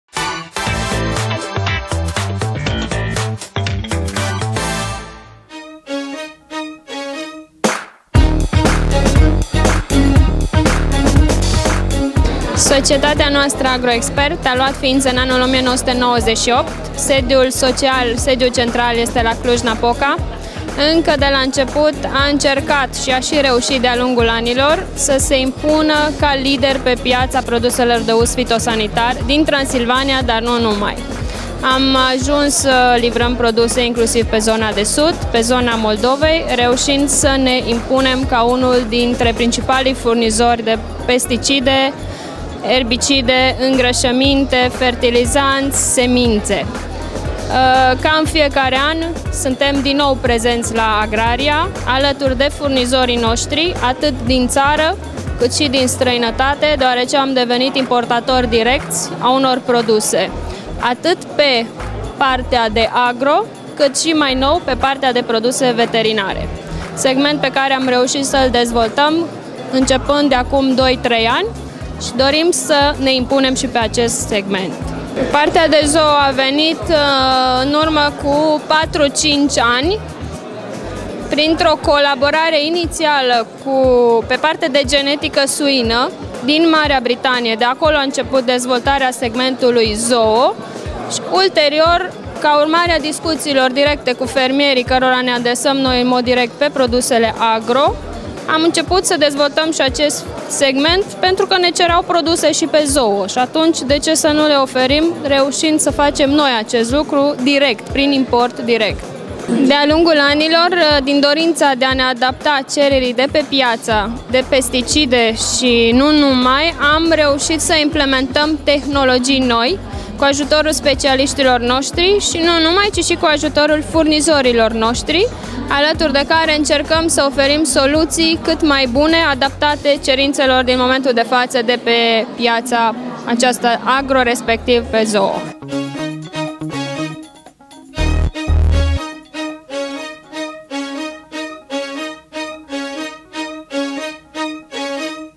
Agroexpert la AGRARIA 2011
Agroexpert la AGRARIA 2011- interviu cu Ioana Achim, reprezentant Agroexpert